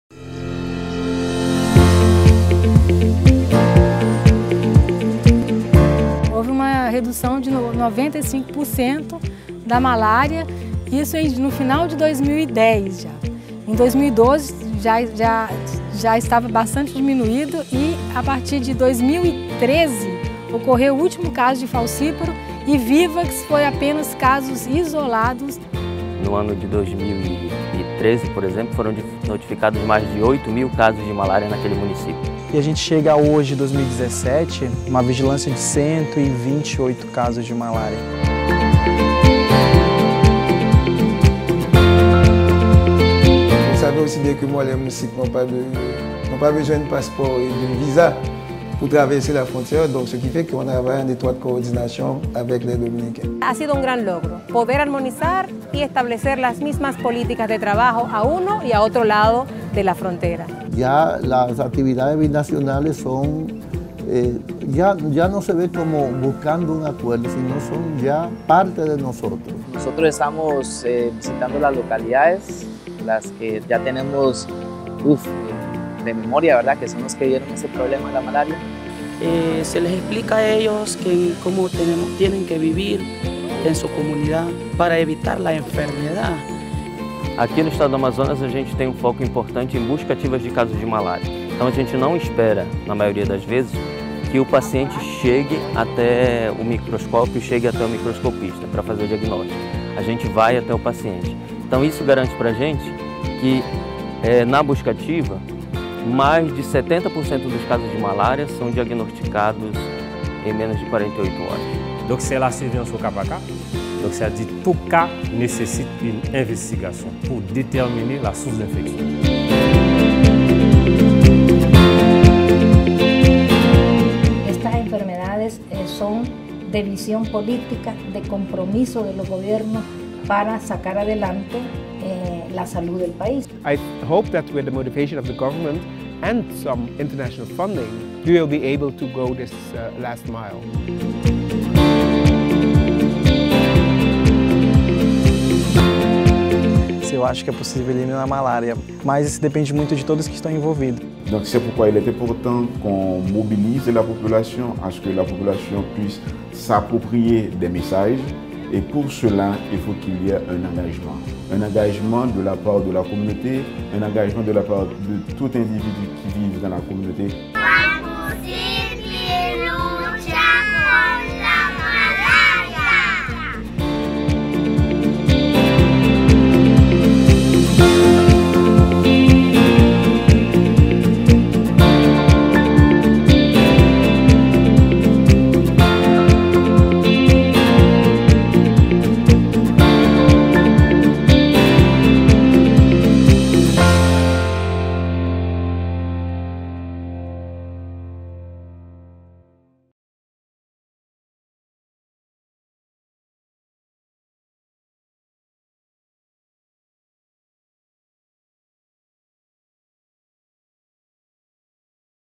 Malaria in the Americas
The Region of the Americas has come a long way in its efforts to curb the burden of malaria since 2000. This video provides a look at the progress made in the Americas and highlights the Pan American Health Organization's Malaria Champions of the Americas.